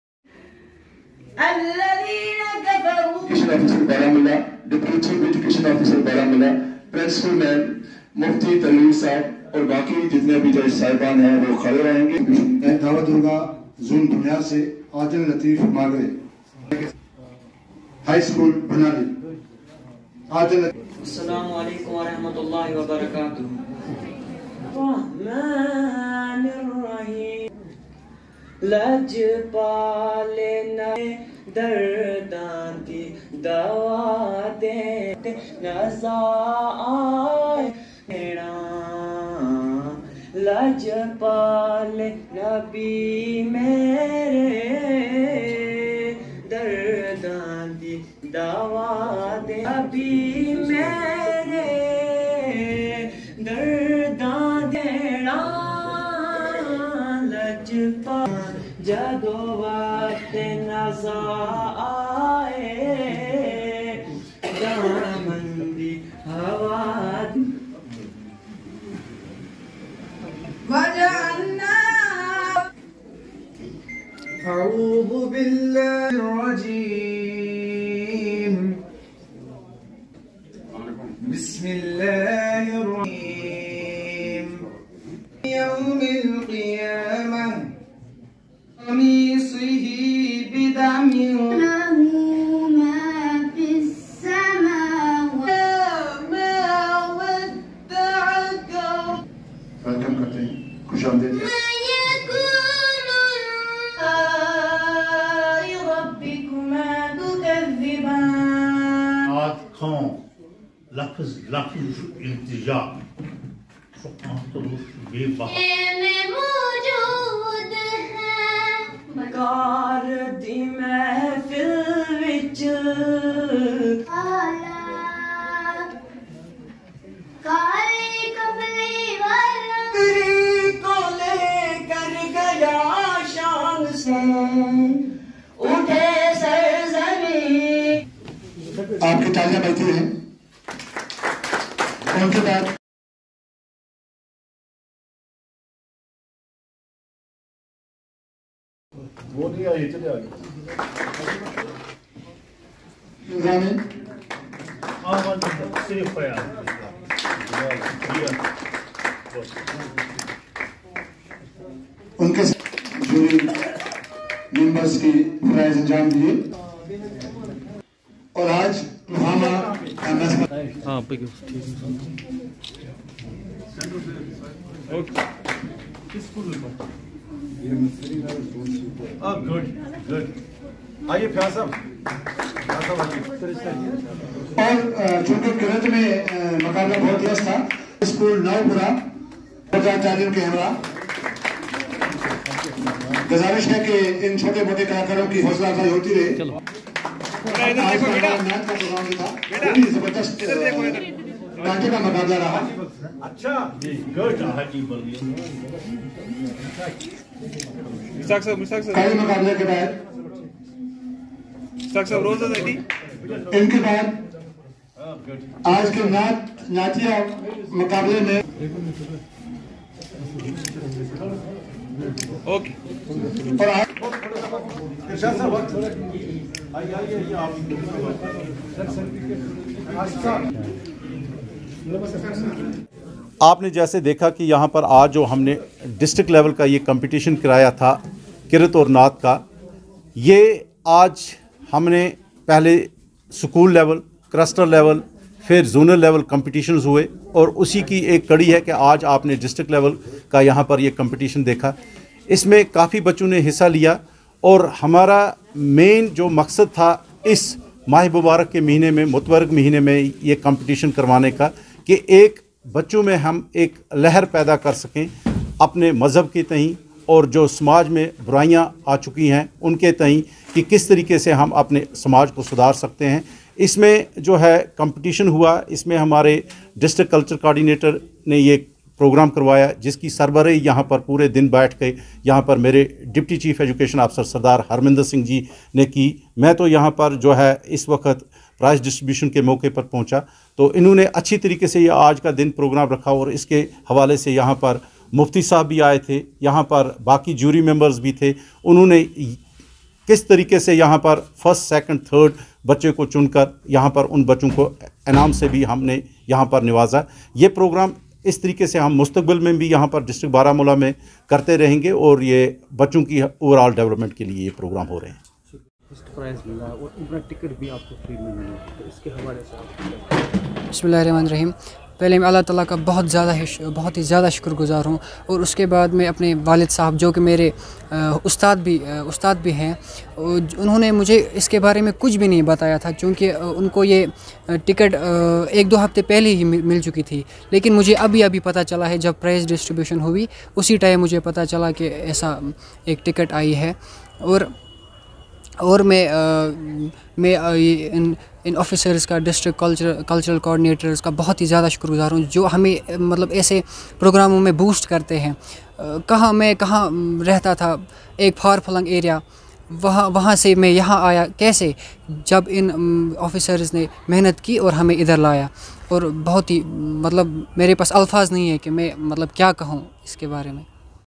District level competition of Naat and Qirat was conducted by Chief Education office Baramulla
District level competition of Naat and Qirat was conducted by Chief Education office Baramulla under Ramzan Special Program series.